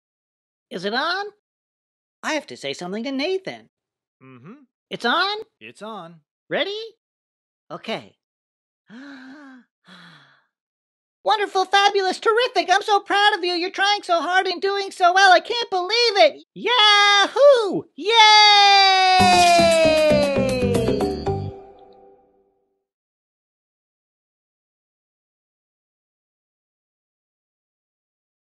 Nathan's message from Buddy
A free, personalized message for a toddler/child with autism!